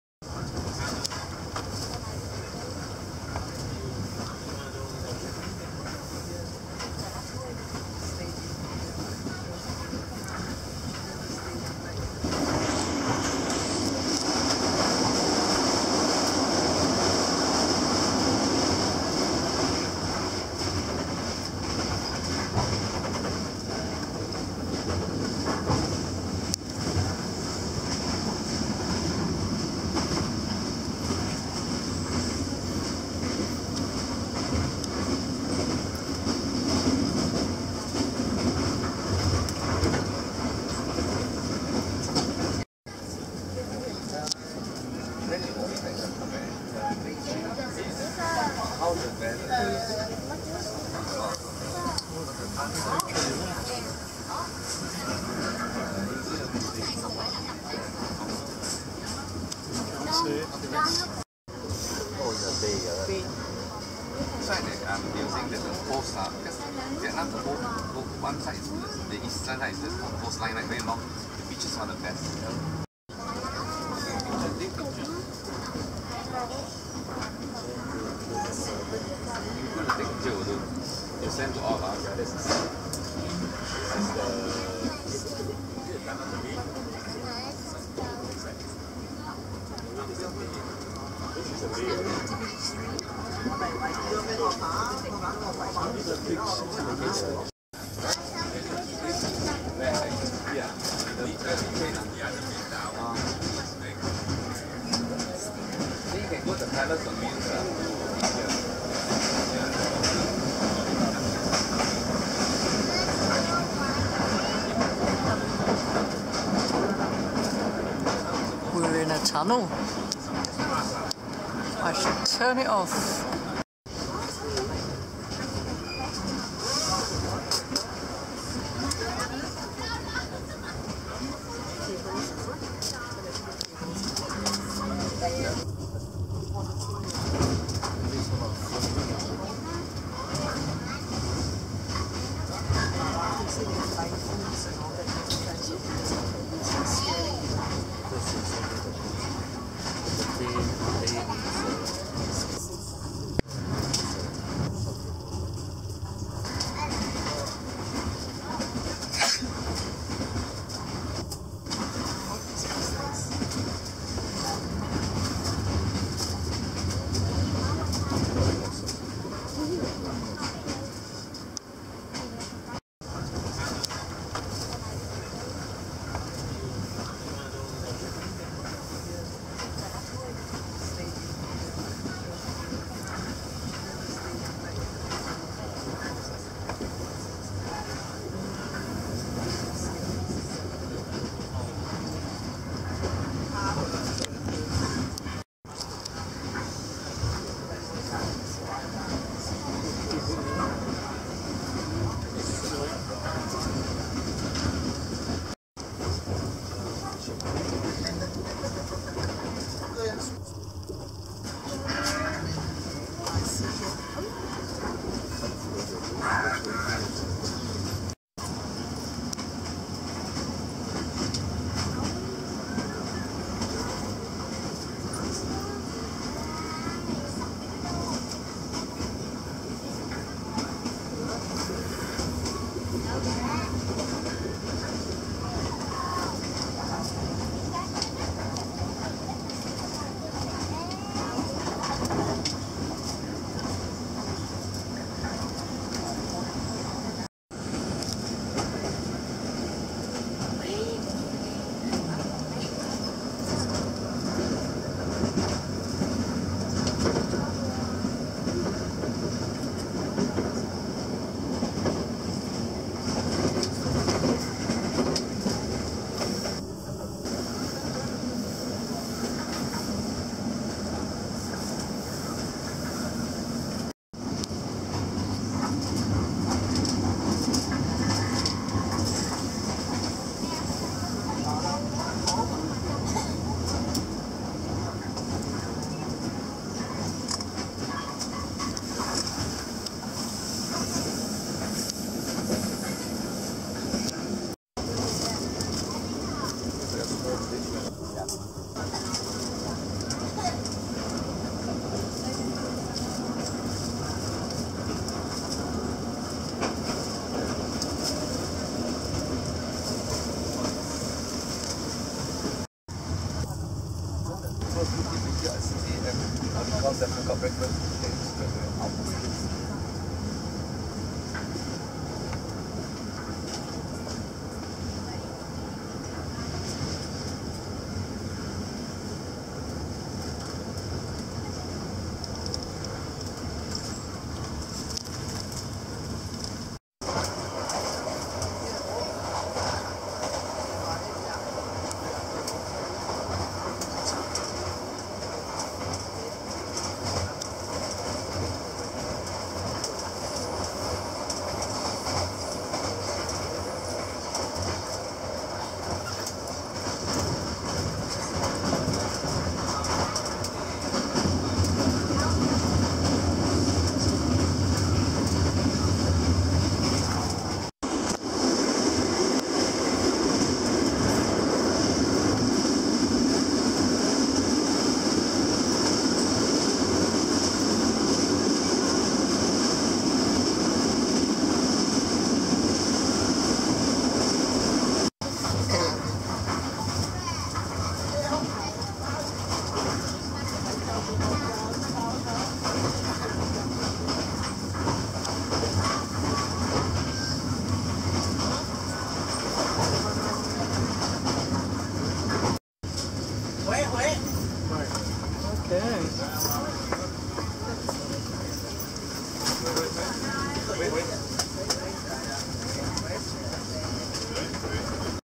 Notdunroamin: Train from Da Nang to Hue, Vietnam
Train journey from Da Nang to Hue, Vietnam.